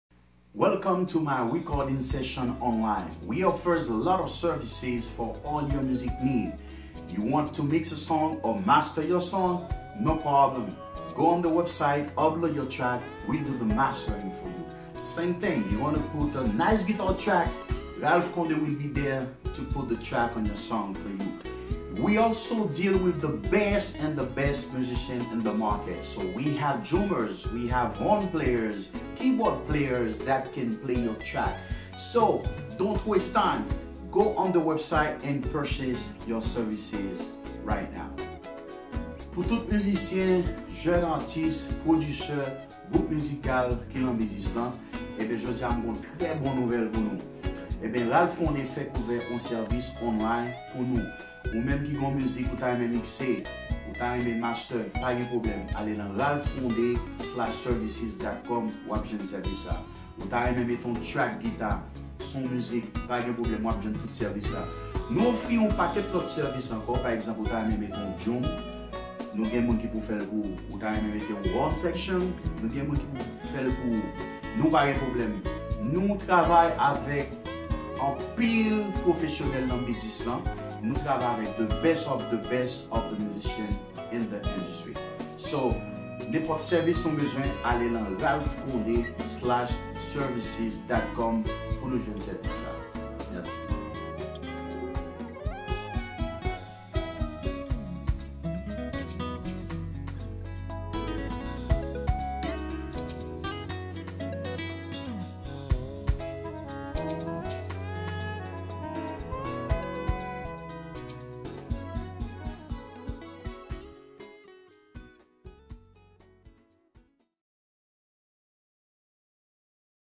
Recording session online With Ralph Conde
we offer now great service online now for your Music recording
Mastering,Mixing,guitar or bass track, drum track ,Horn Session and more .He have all the best professional musician in the music business to play on your track.
Start and purchase your session today